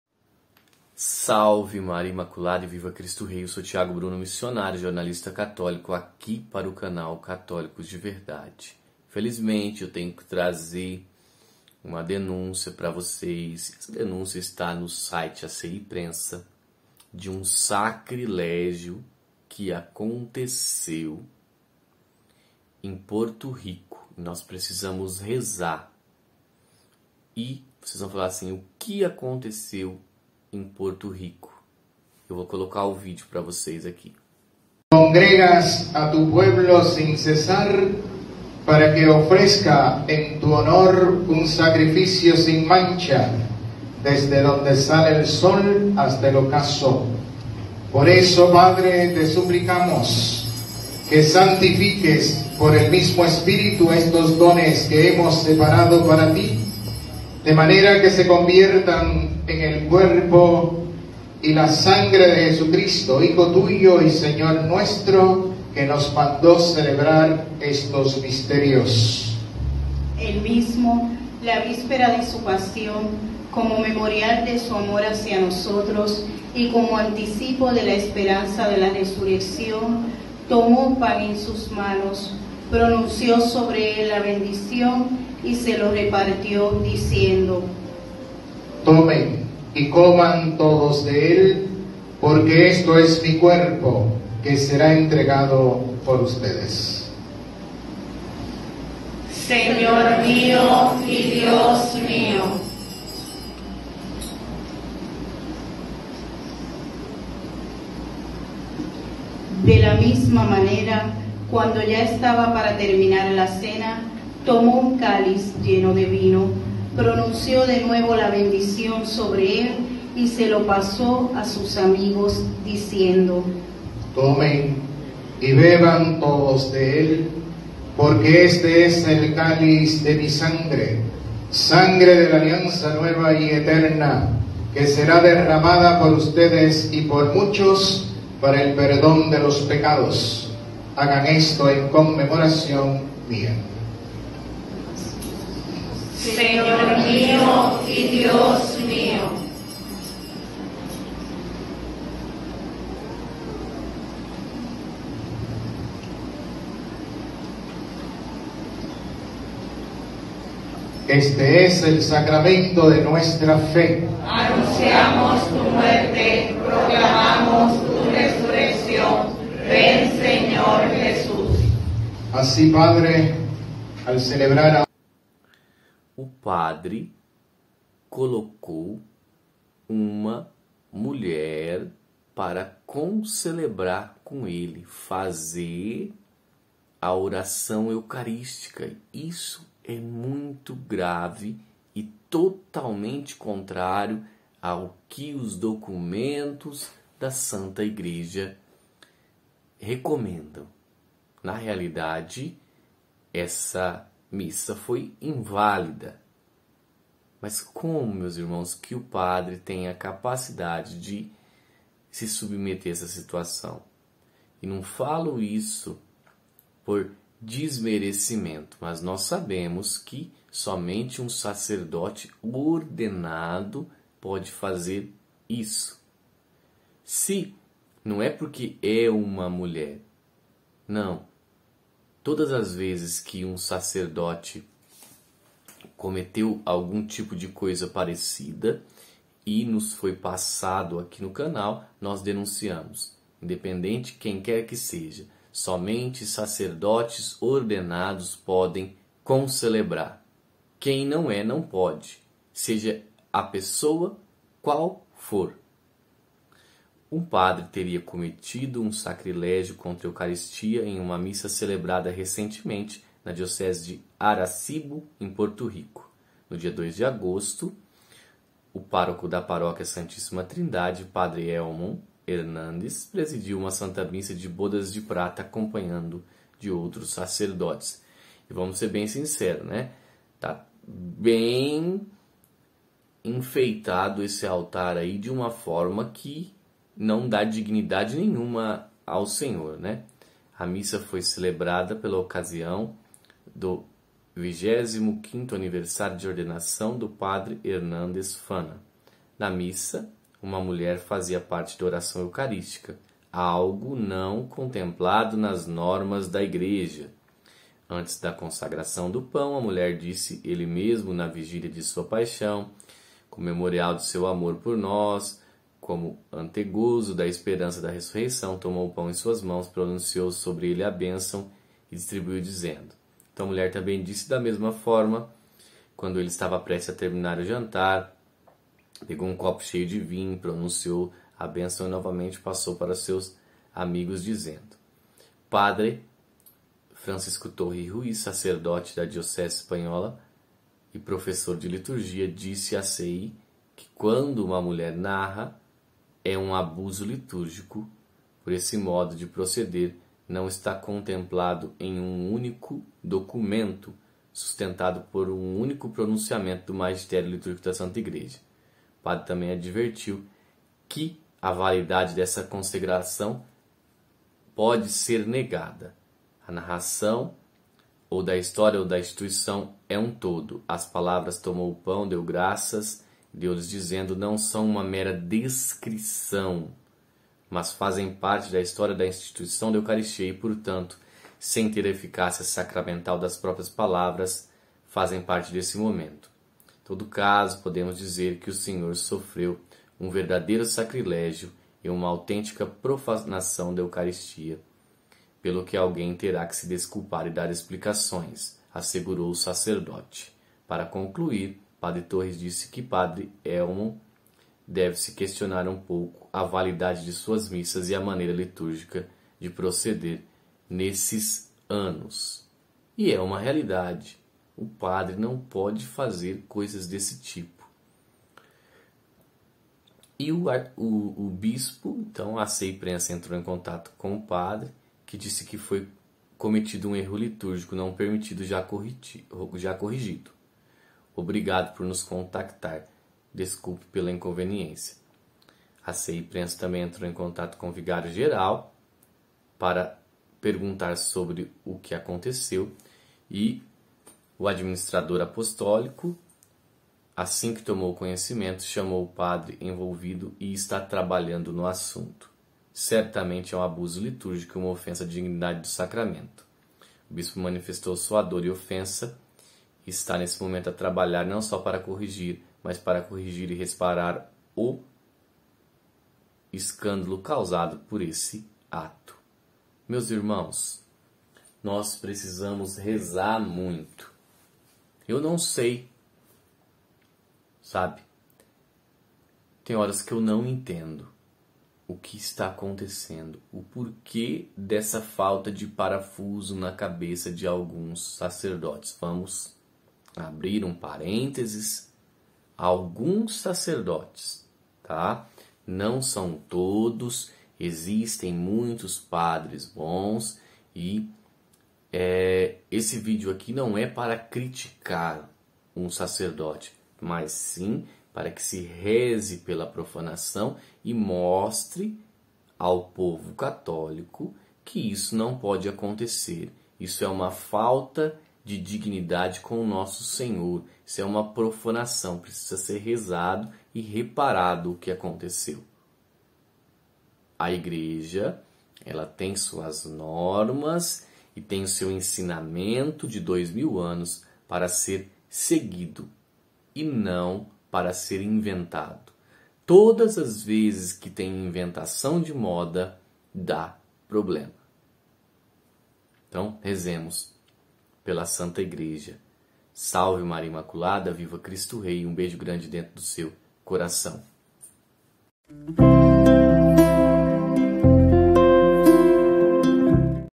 URGENTÍSSIMO!! O PADRE SEM PARAFUSO COLOCOU UMA LEIGA PARA CONCELEBRAR A MISSA!!
NOTÍCIAS DA IGREJA CATÓLICA.
Fonte Aciprensa...